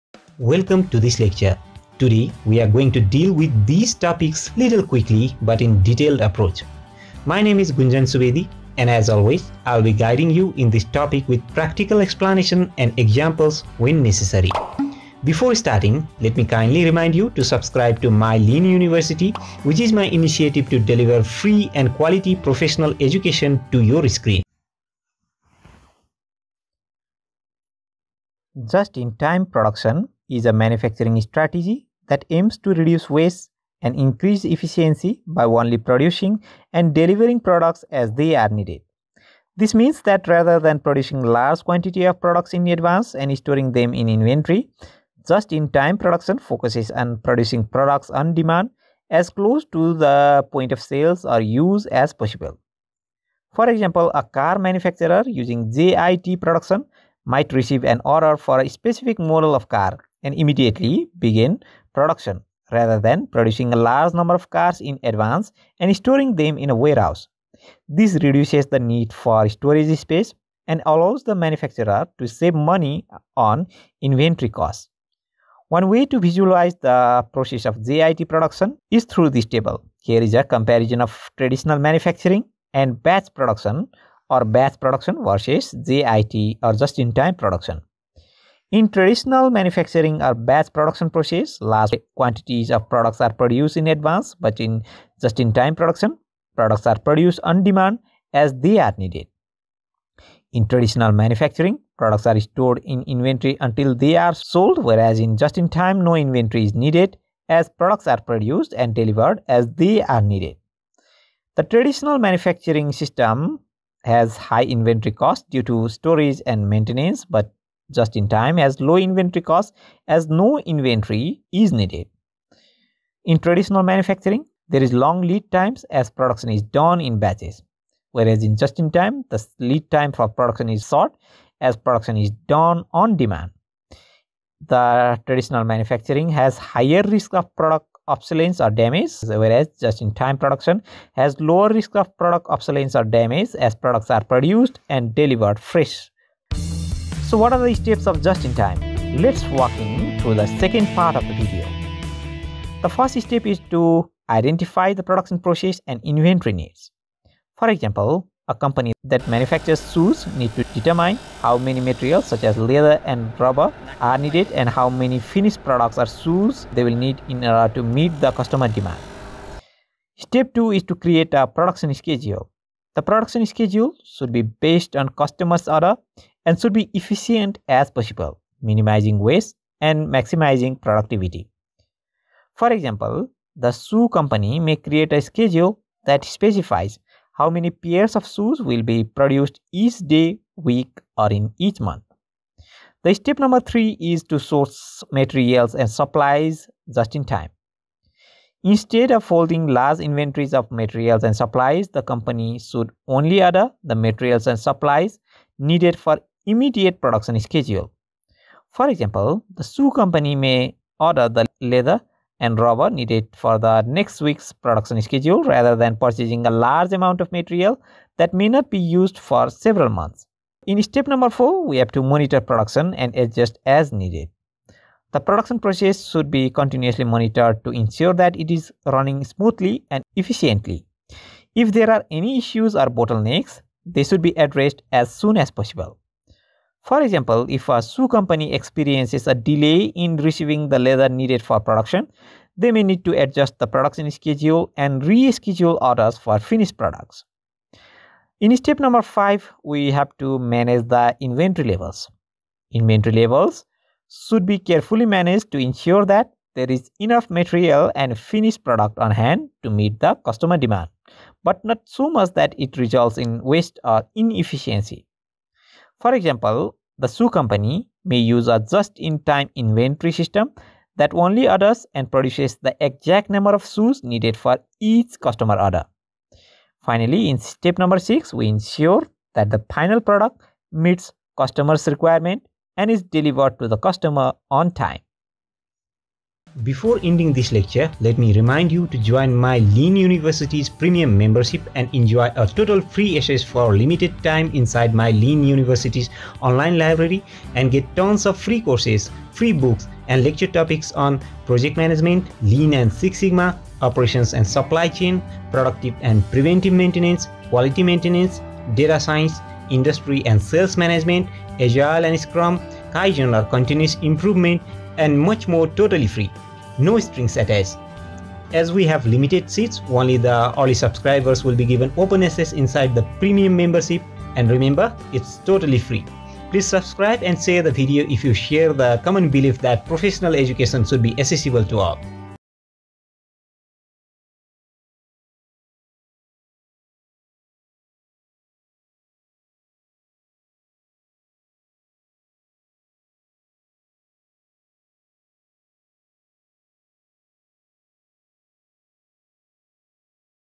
Just In Time Process | Step-by-Step Guide with Just in Time ( JIT ) examples
Welcome to our in-depth tutorial on implementing the Just In Time (JIT) process in your operations! In this video, we'll walk you through the essential steps of JIT, a lean manufacturing methodology that emphasizes efficiency, waste reduction, and timely production.

Step 1: Identify the production process and inventory needs

Determine the materials and finished products required to meet customer demand.

Step 2: Create a production schedule

Develop an efficient schedule based on customer orders to minimize waste and maximize productivity.

Step 3: Source materials and supplies just in time

Order materials and supplies as needed for immediate production, avoiding excess inventory.

Step 4: Monitor production and adjust as needed

Continuously monitor the production process to address any issues or bottlenecks promptly.

Step 5: Manage inventory levels

Maintain optimal inventory levels to meet customer demand without excess waste.

Step 6: Ensure the final product meets customer requirements and is delivered on time

Verify that the finished product meets quality standards and deliver it to the customer as scheduled.
Implementing these steps effectively can help organizations streamline their production processes, reduce waste, and improve overall efficiency in Just-in-Time production systems.

Join us as we delve into the world of Just In Time manufacturing and discover how it can revolutionize your operations. Whether you're a seasoned industry professional or a newcomer to lean principles, this tutorial is designed to equip you with the knowledge and tools to succeed in implementing Just In Time effectively.